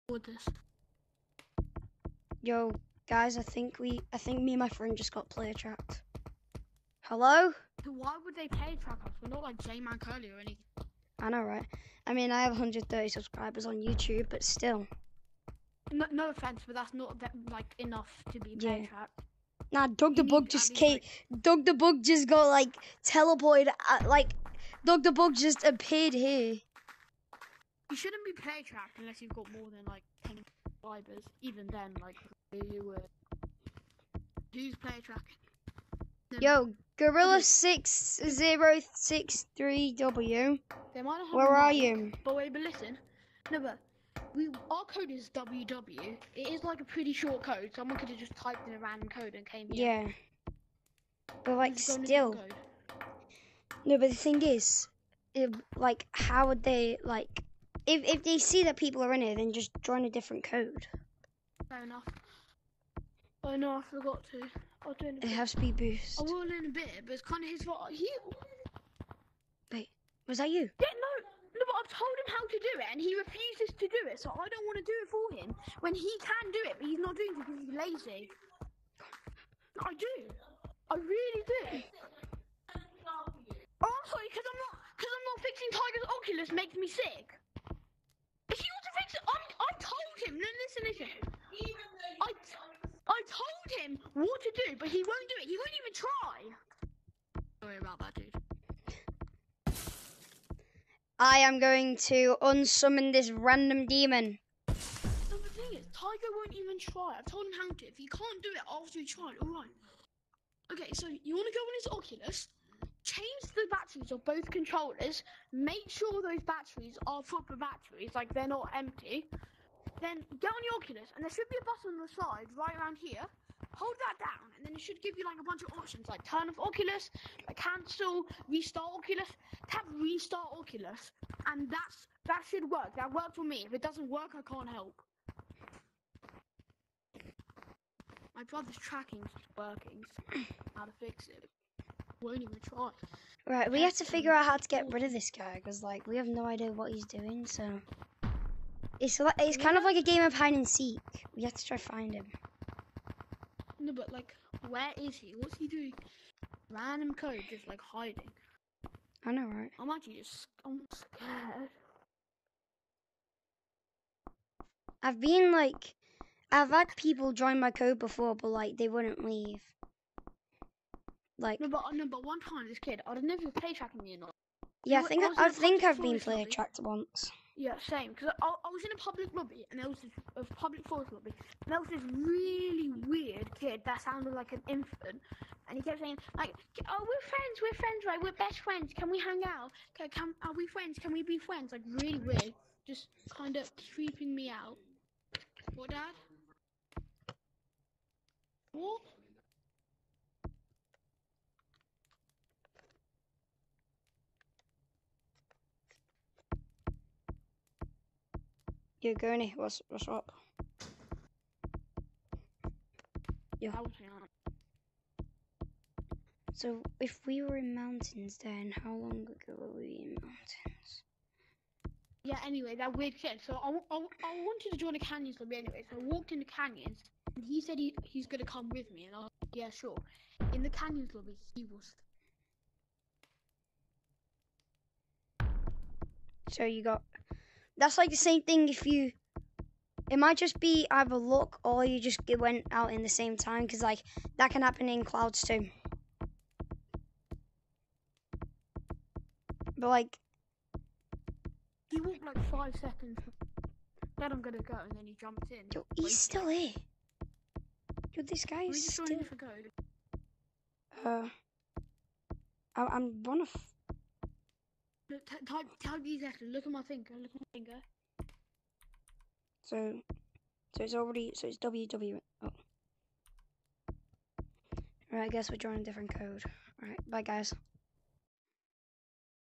I think i got player tracked (ft GONY)
we joined a private code (WW) and we were trying to do the group join till we realised 3 people were in the code and we went out searchung for him